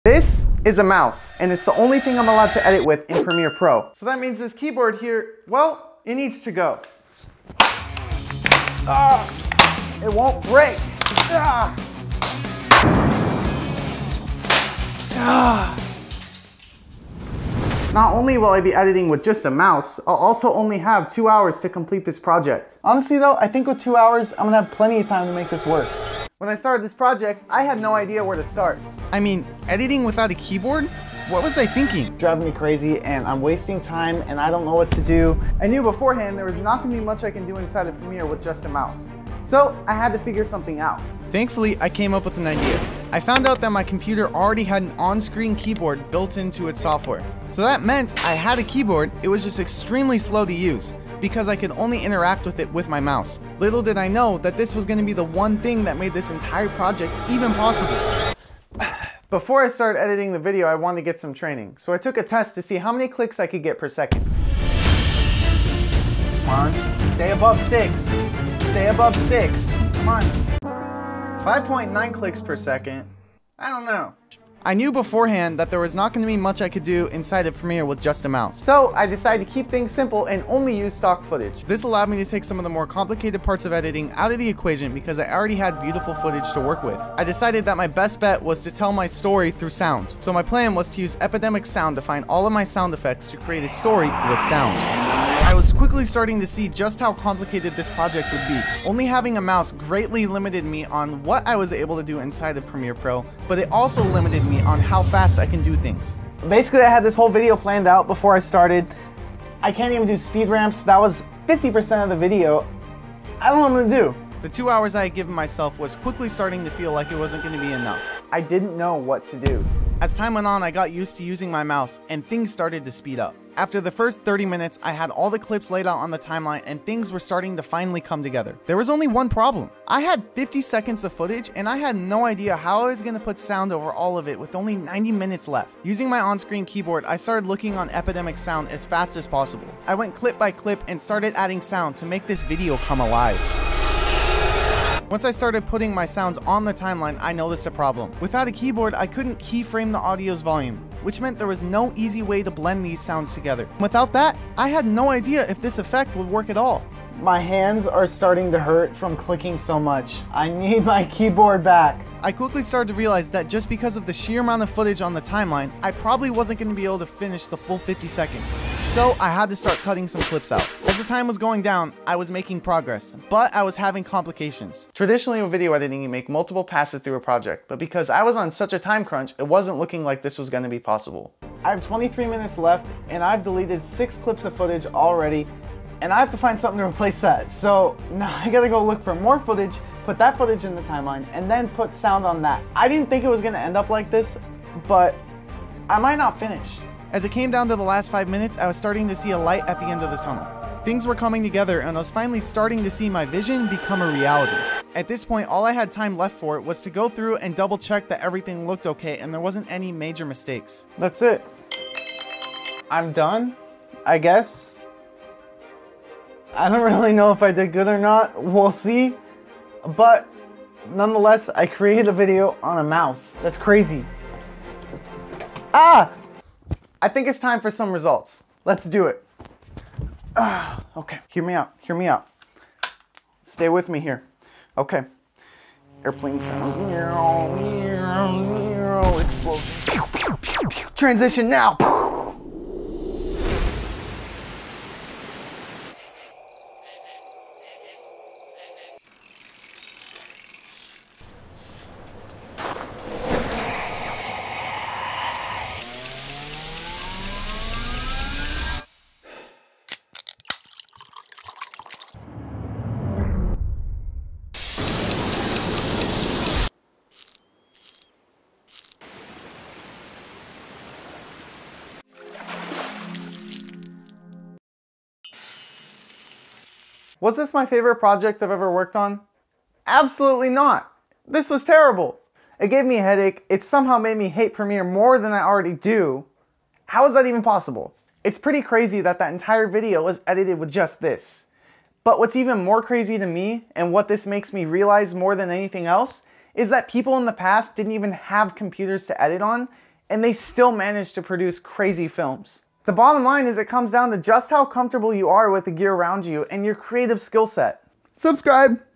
How I Made An Entire Video Only Using A Mouse.
In this video, we're putting my video editing skills to the test! Can I edit a full length film using only a mouse? It's going to be a wild ride filled with challenges and lots of clicks! With my limited resources, will I be able to create a visually stunning film that tells a captivating story? Or will I be left with a messy, unorganized video that makes no sense? Watch to find out!